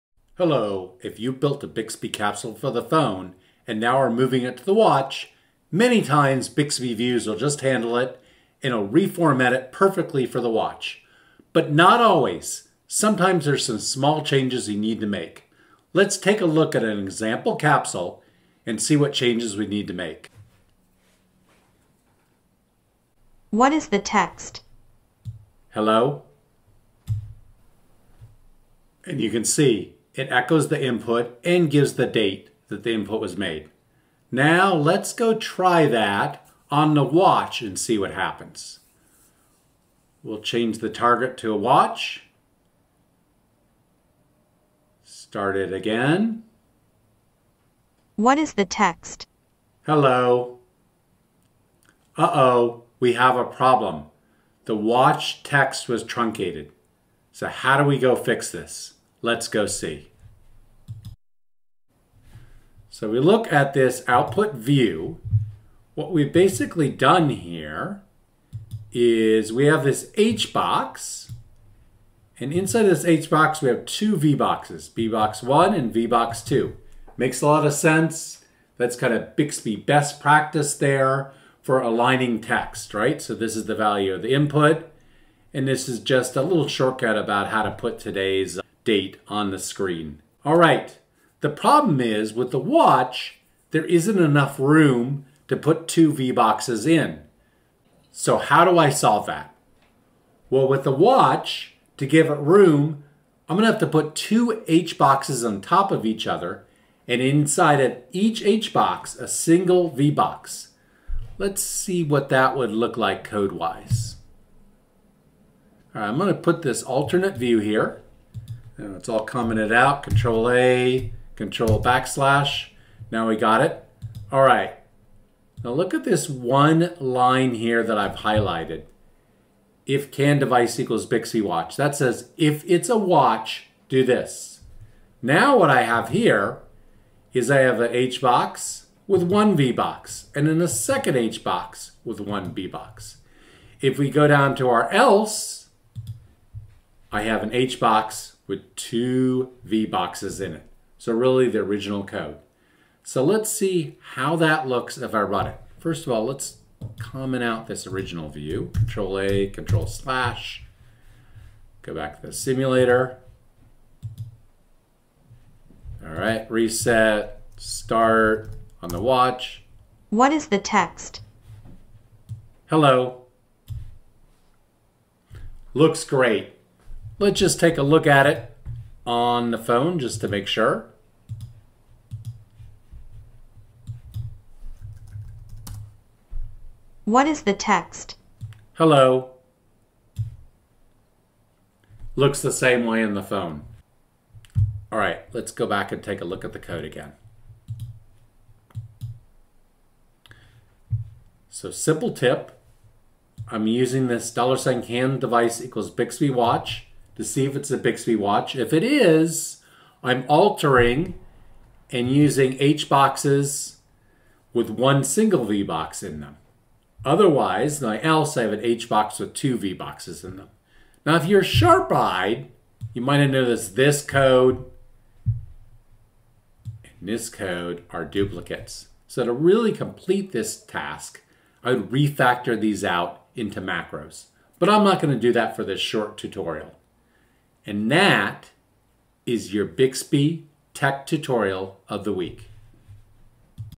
Adjusting Views for the Watch - Bixby Tech Tutorial of The Week
Most capsules designed for the phone will work great on the watch but in some cases you need to make a few tweaks. The most common one is altering hbox and vbox code - this tutorial shows you an example of a simple change that optimizes the watch UI